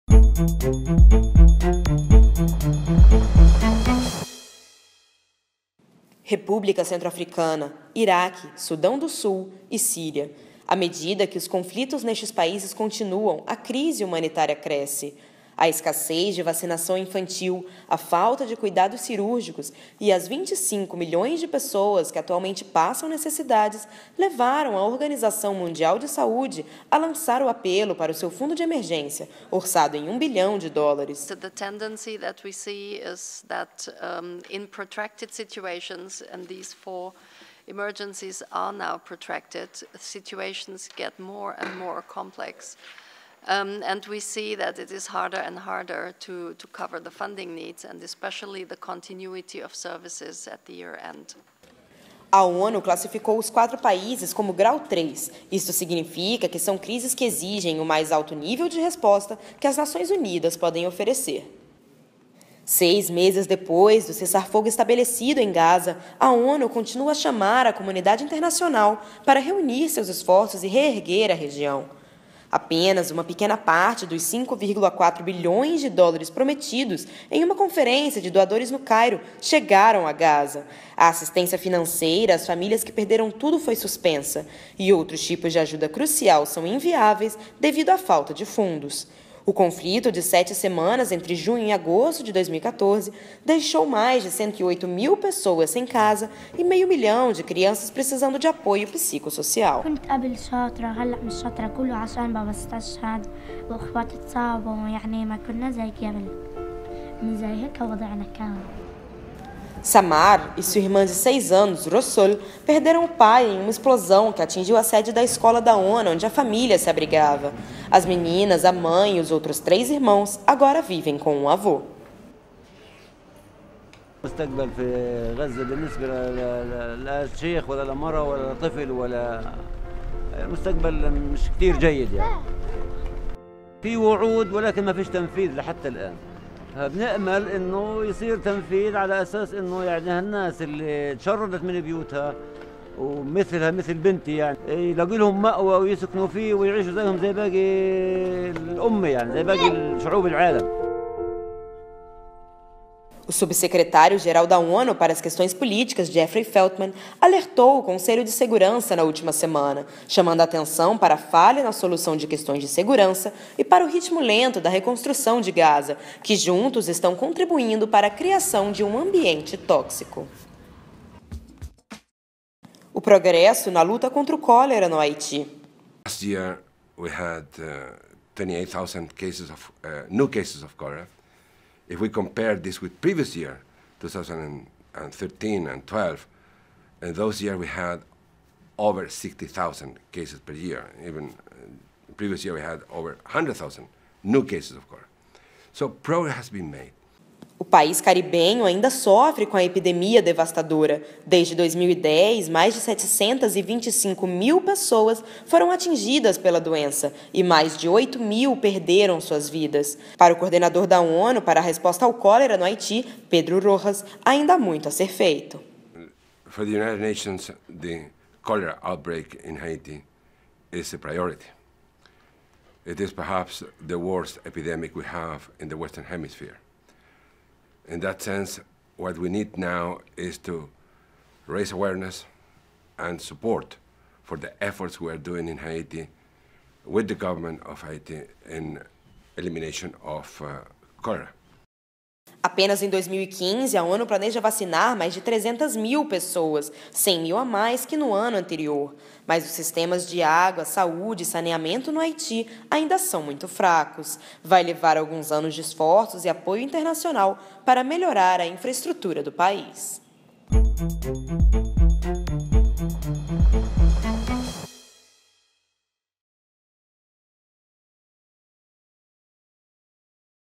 Resumo semanal da ONU em imagens #31
O apelo humanitário da Organização Mundial da Saúde (OMS) para 25 milhões de pessoas em necessidade; os recursos prometidos e não efetivados para Gaza; o progresso na luta contra o cólera no Haiti – estes são os destaques do resumo semanal da ONU em imagens, edição número 31.